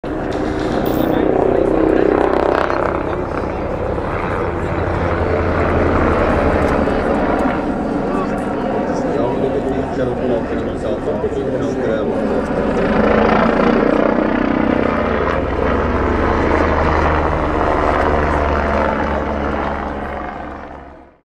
ZigAirMeet Flight Noise Teaser «MBB Bo-105C»
Emotion pur in den Glarner Bergen mit klopfenden Rotoren und einer geballten Ladung Action über dem Himmel von Mollis am 18. und 19. August 2023: Sei dabei am ZigAirMeet 2023!

Pure emotion in the Glarus mountains with beating rotors and a concentrated load of action over the skies of Mollis on August 18 and 19, 2023: Be there at the ZigAirMeet 2023!